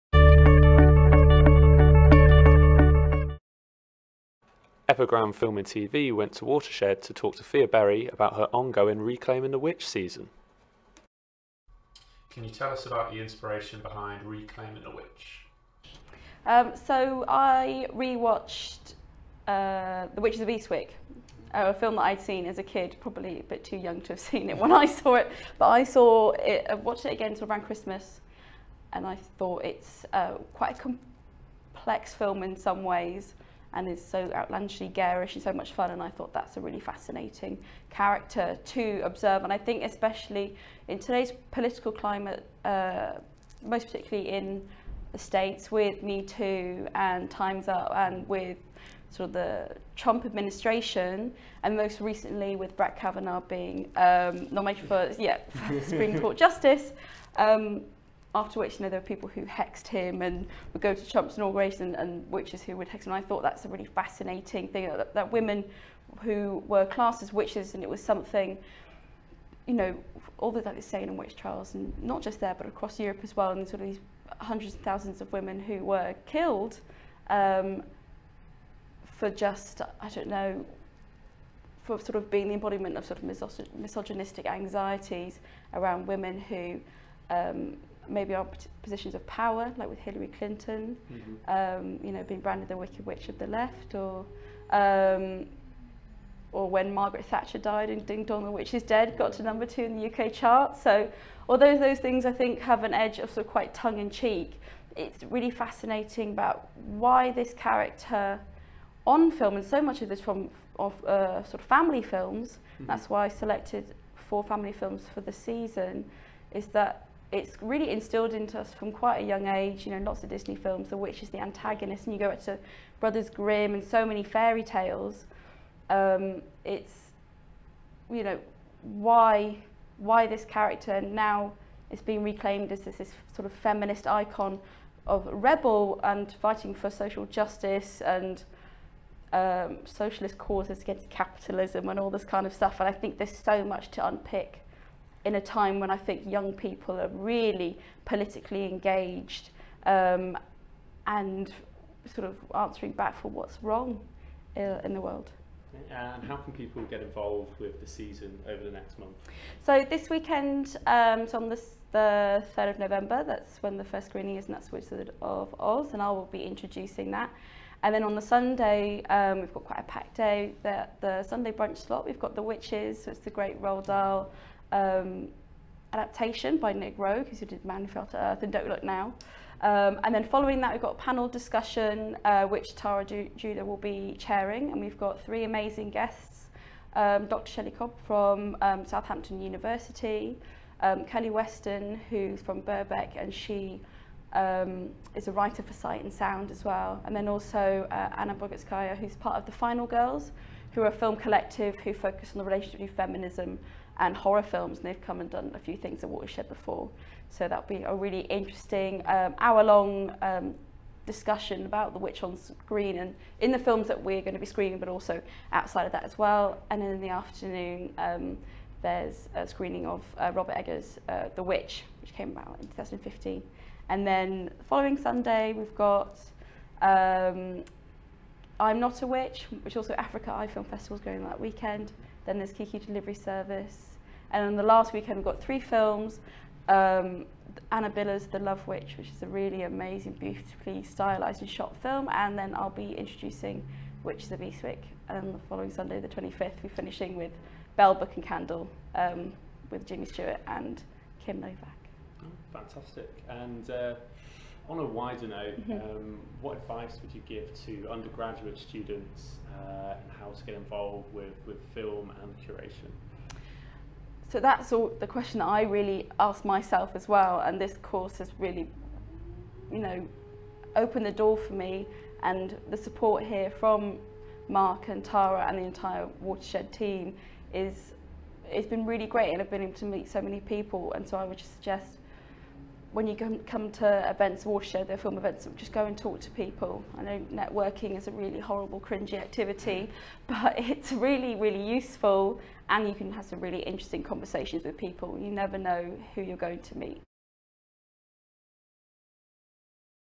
Epigram talks to: Thea Berry about her project: "Reclaiming the Witch".
Epigram Film and TV spoke to Thea Berry at the Watershed cinema in Bristol.

She spoke about her project "Reclaiming the Witch", and it's connections to modern day culture, Feminism and Trump!